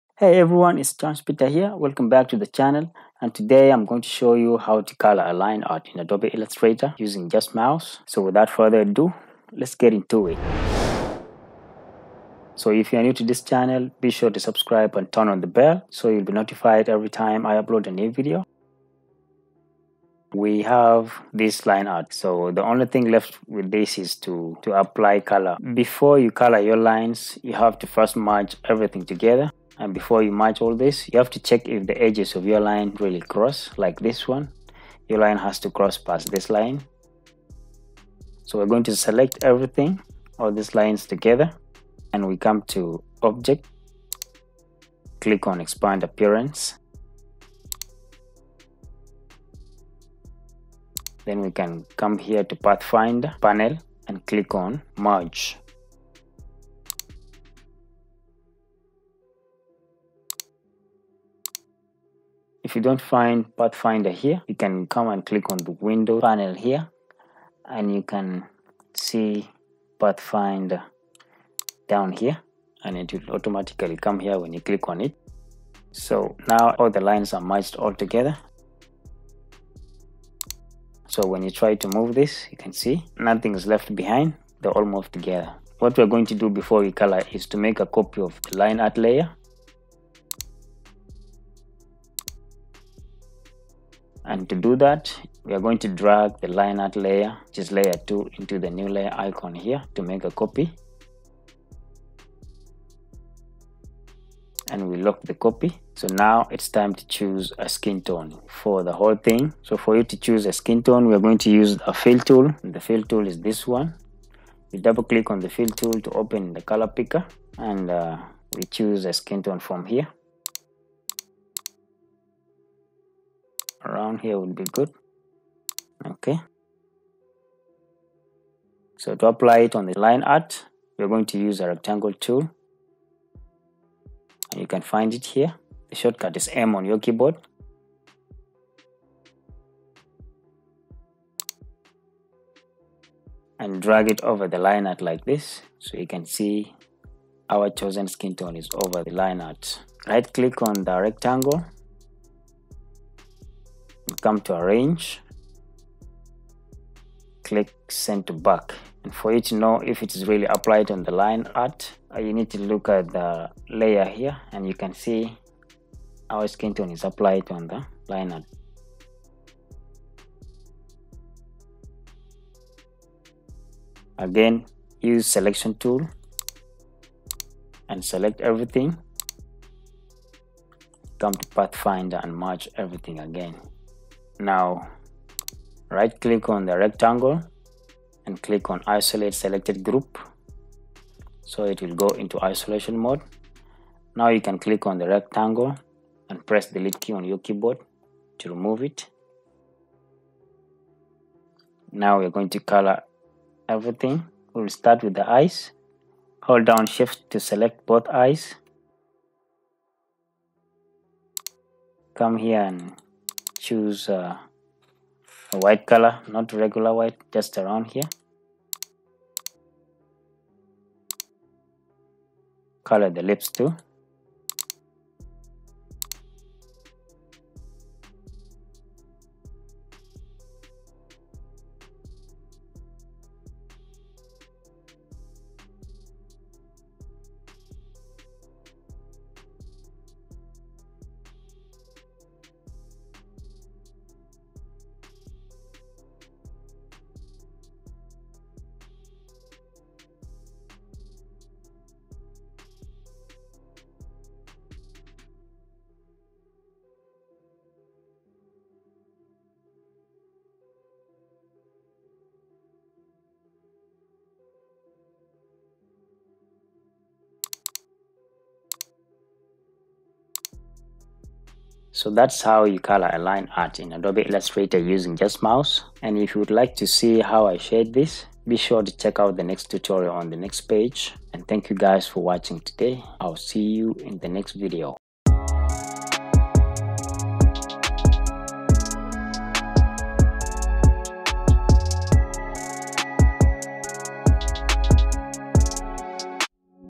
COLOR Your LINEART(SKETCH) Like a PRO - Adobe Illustrator [step-by-step] tutorial
In this video tutorial, I will teach y'all how to color vector line art like a Pro in adobe illustrator step-by-step using just mouse💻🖱🖌💯.
There are a combination of tools that you can use to color with e.g. the Selection & Direct Selection tool respectively. Enjoy💻🖱🖌🤗

GET YOU SOME NICE GEAR/EQUIPMENTS

✅Wireless Drawing Mouse🖱
✅Ergonomic Gaming Mousepad
✅Recording Microphone